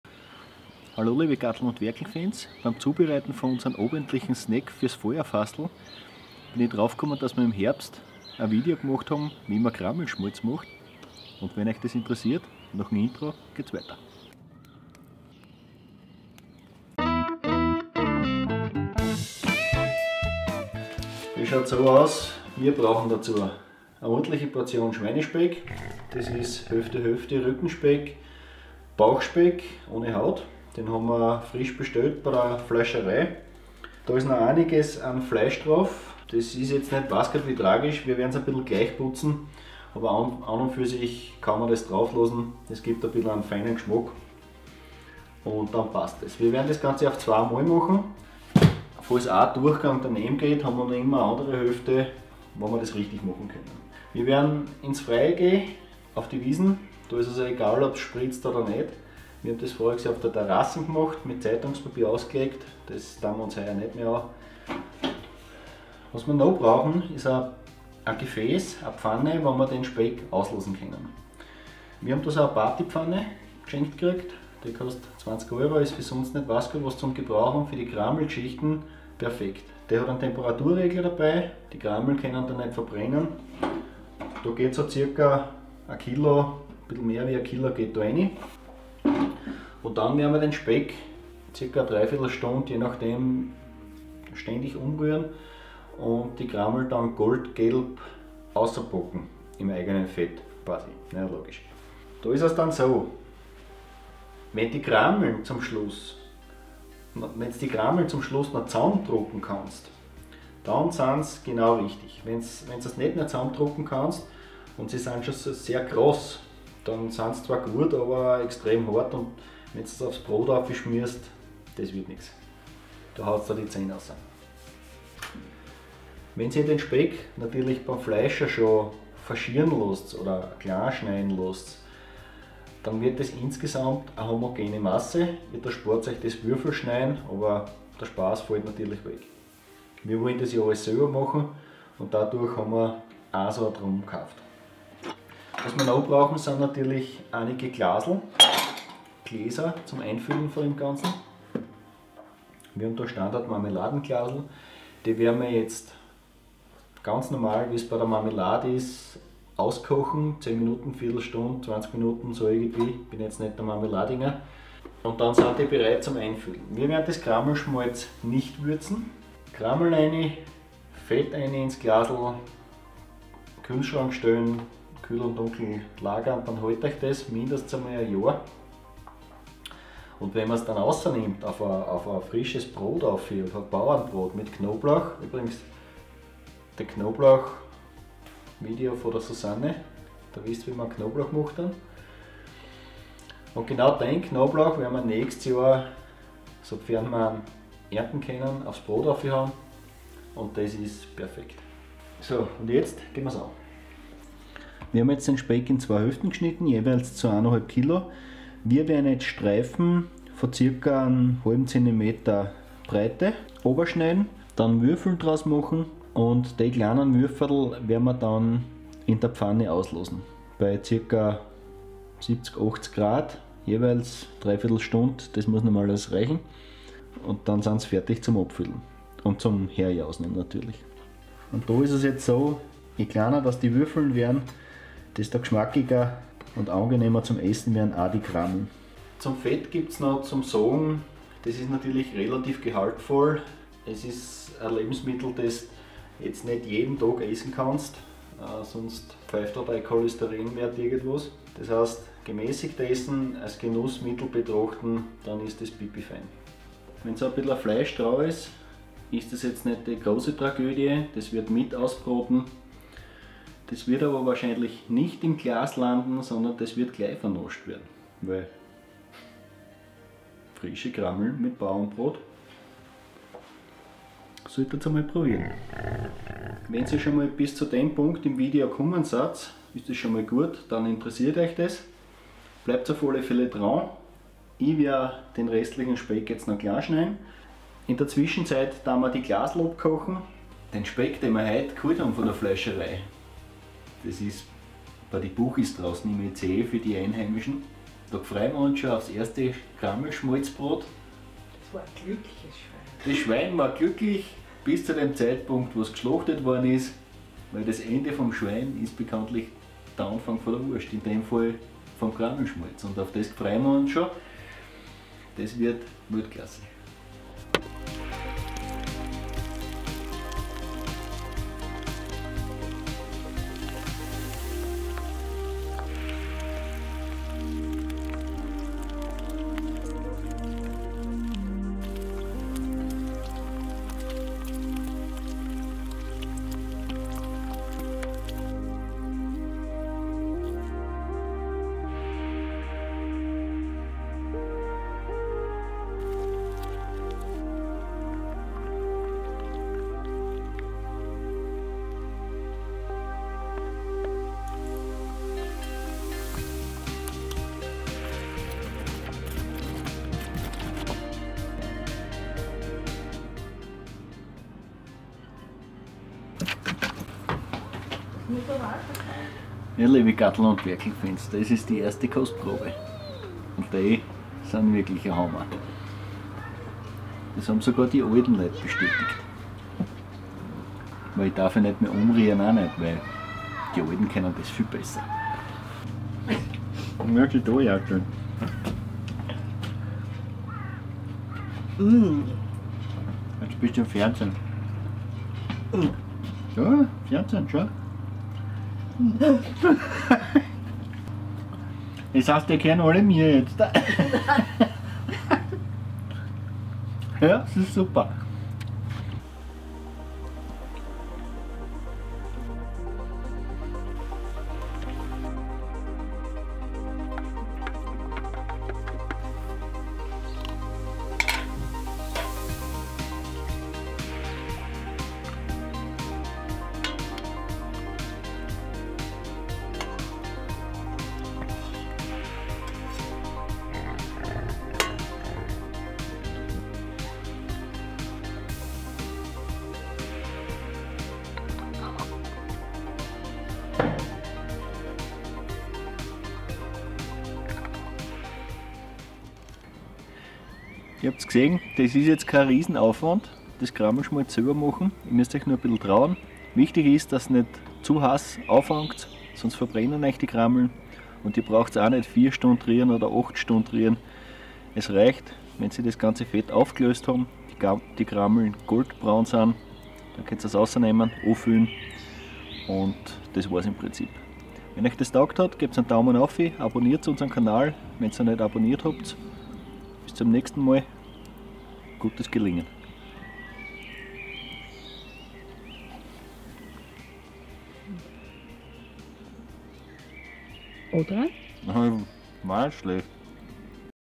Grammelschmalz selbermachen. Griebenschmalz selbermachen
In diesem Video geht es darum, wie du dein Grammelschmalz oder Griebenschmalz oder Flomenschmalz selber machen kannst. Es geht recht einfach, du brauchst nur ein wenig Geduld.